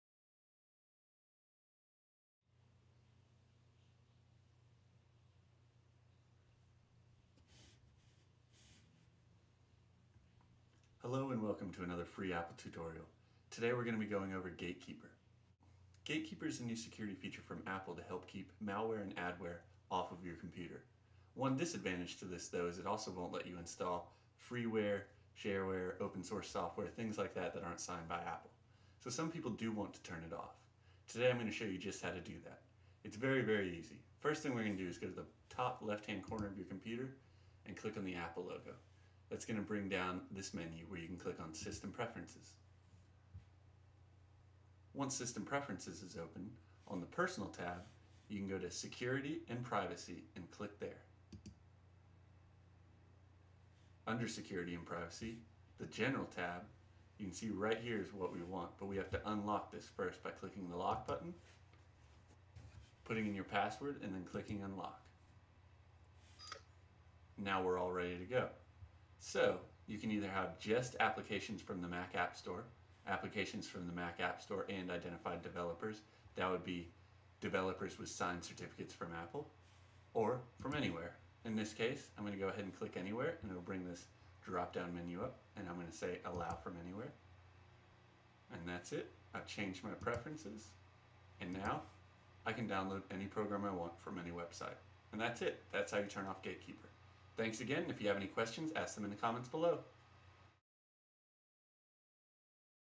OSX Mountain Lion: How To Disable Gatekeeper
Gatekeeper is a new feature in Mountain Lion that builds on OS X's existing malware checks to help protect your Mac from malware and misbehaving apps downloaded from the Internet.

Although having Gatekeeper enabled keeps you safe, it will not allow you to install some third party applications. If you want to install open source software or third party applications you will need to disable Gatekeeper.

OSX Mountain Lion
2012